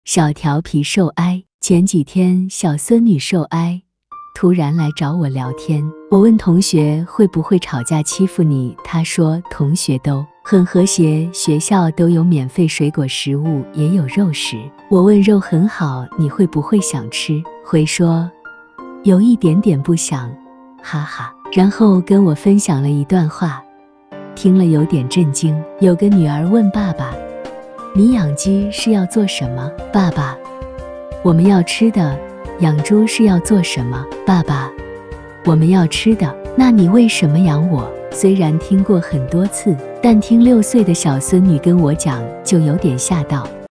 【小調皮瘦欸】
前幾天小孫女【瘦欸】突然來找我聊天
我問同學會不會吵架欺負妳 她說同學都很和諧 學校都有免費水果食物 也有肉食
我問肉很好吃妳會不會想吃？
回說：有一點點不想 哈哈
然後跟我分享了一段話：聽了有點震驚
有個女兒問爸爸：你養雞是要做什麼？
爸爸：我們要吃的。
養豬是要做什麼？
爸爸：我們要吃的。
【那你為什麼養我？？？！！！】
雖然聽過很多次
但聽六歲的小孫女跟我講就有點嚇到